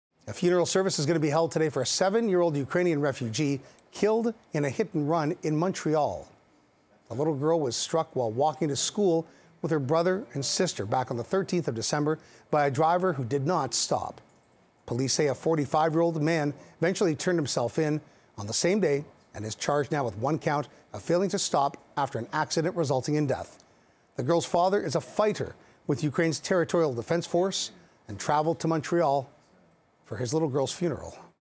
Funeral today for young Ukrainian refugee struck and killed in Montreal
A funeral service will be held today for a seven-year-old Ukrainian refugee killed in a hit and run in Montreal.